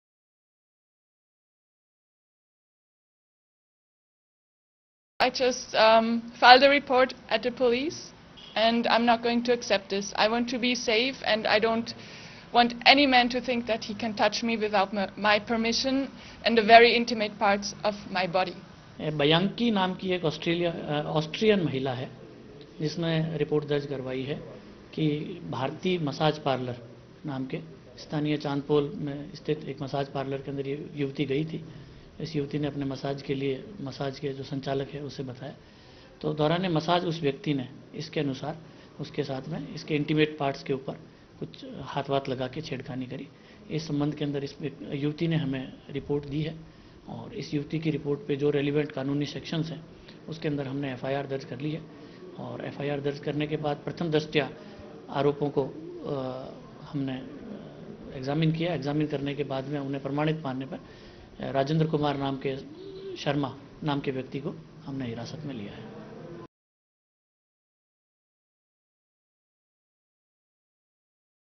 Austrian tourist allegedly molested, accused arrested - Rajasthan News
Udaipur (Rajasthan), Mar 27 (ANI): In a shocking incident, an Austrian woman was allegedly molested by a man in Rajasthan’s Udaipur district on Monday. The police registered a case in the matter and accused was arrested within no time.